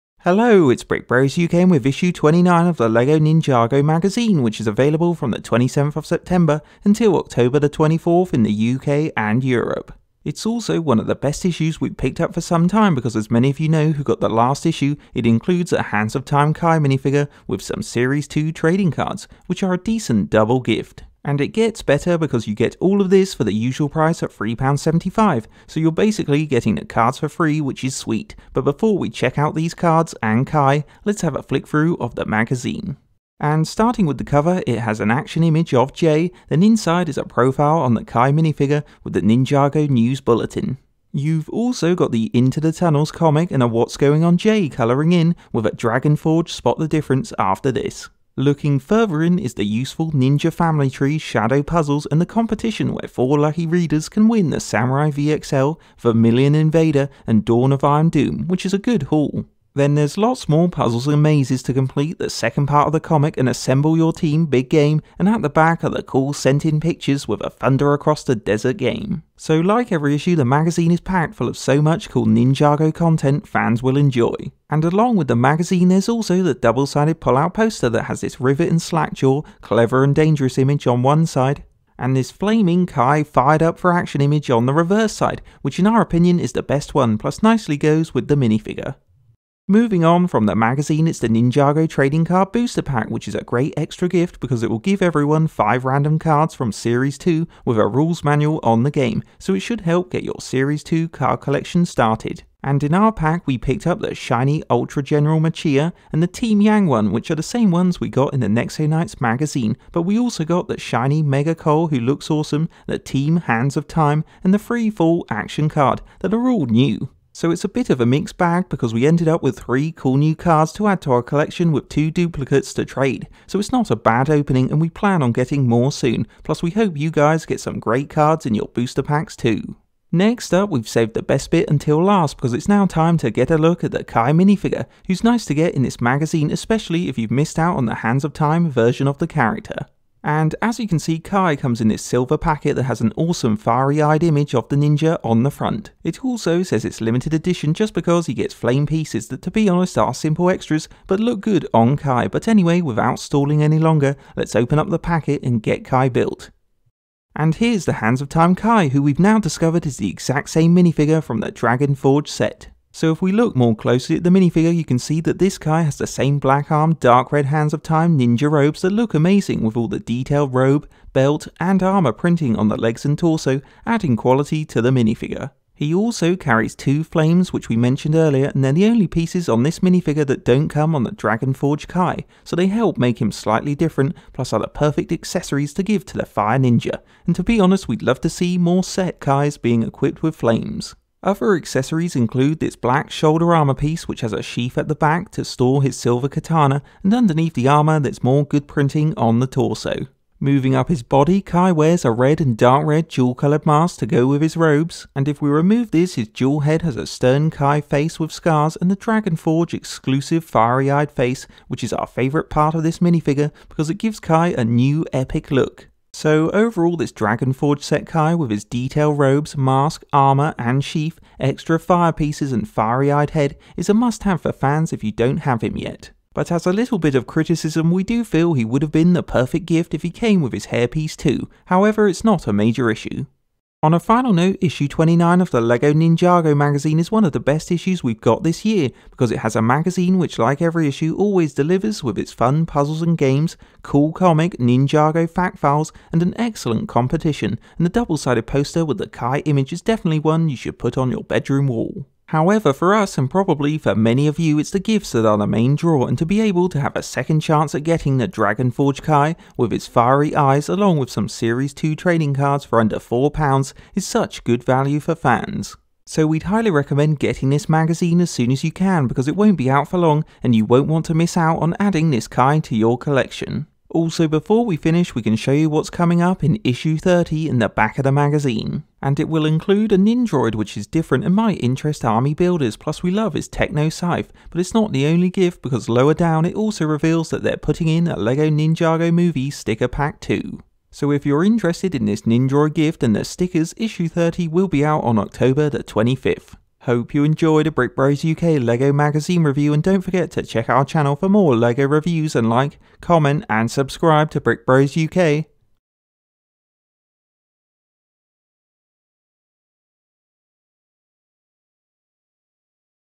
LEGO NINJAGO ISSUE 29 MAGAZINE FIRE KAI MINIFIGURE REVIEW!
Here's Our Review Of Issue 29 Of The Lego Ninjago Magazine Which Includes A Fire Kai Minifigure With Trading Cards!!! Price:-£3.75

Videos That You May Also Be Interested In Below:-